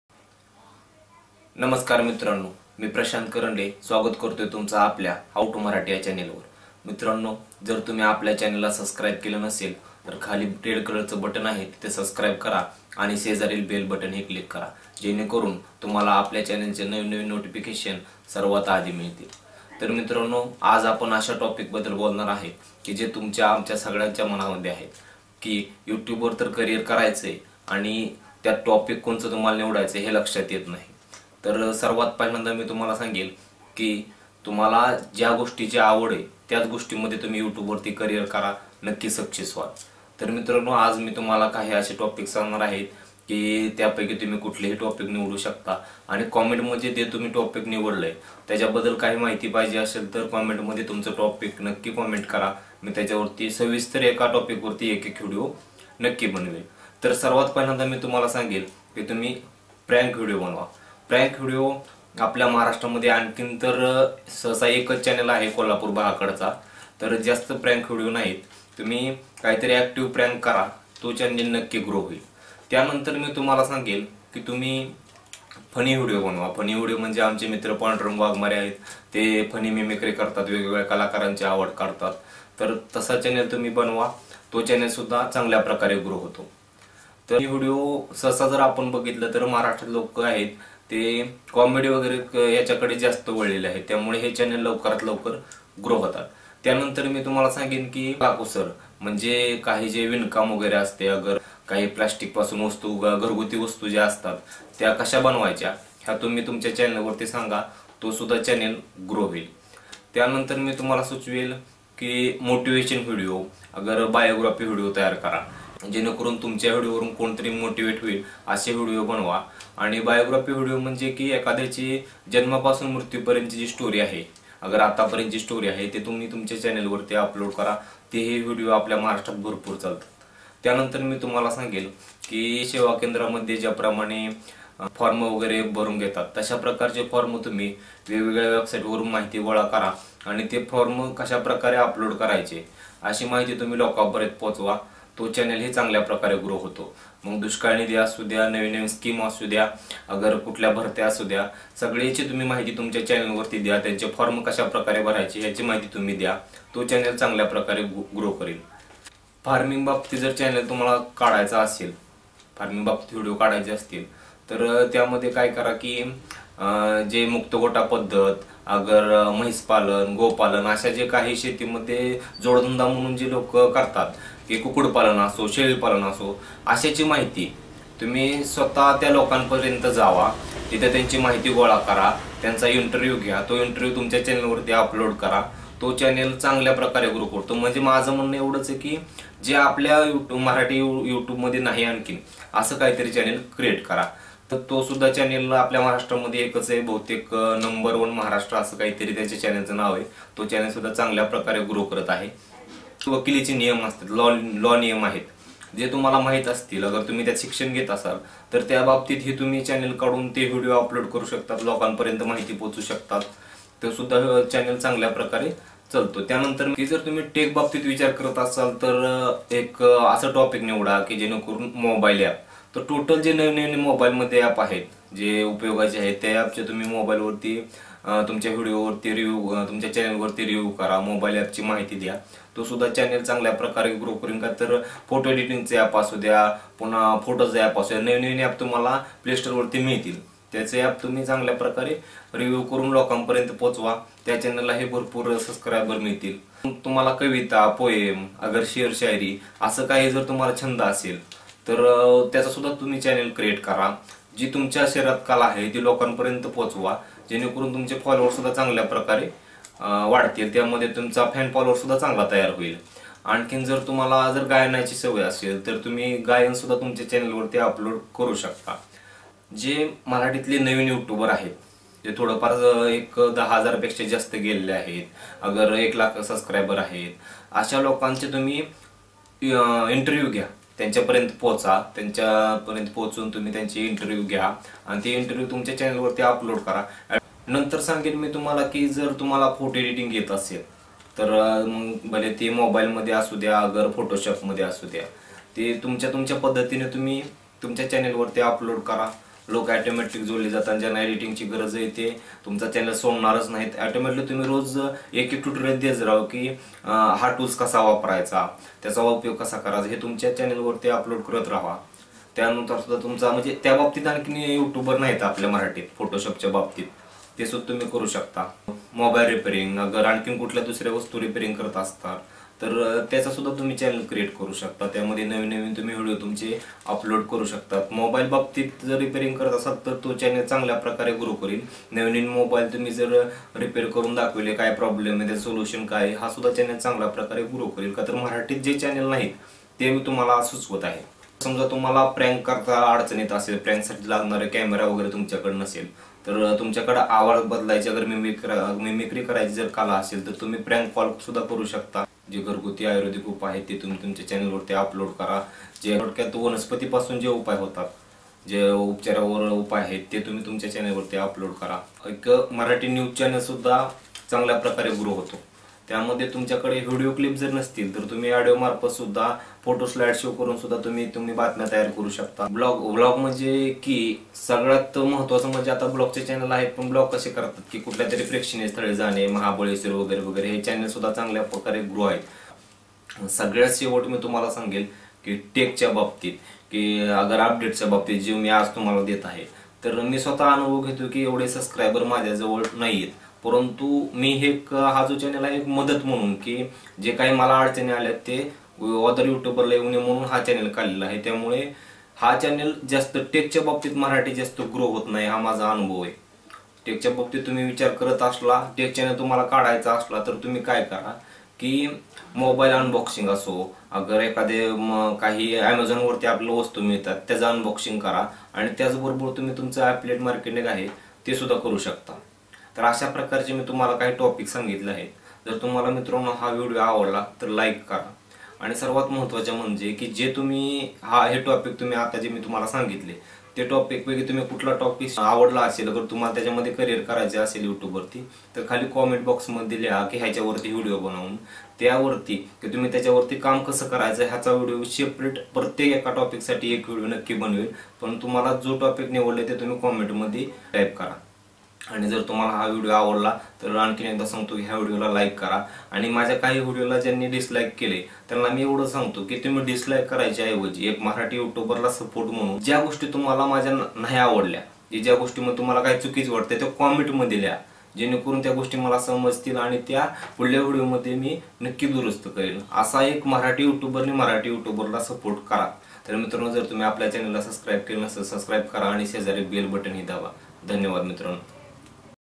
How to select channel topic in Marathi
नमस्कार मित्रांनो आज मी तुम्हाला सांगणार आहे की तुम्ही च्यानेलला टॉपिक कसे निवडाल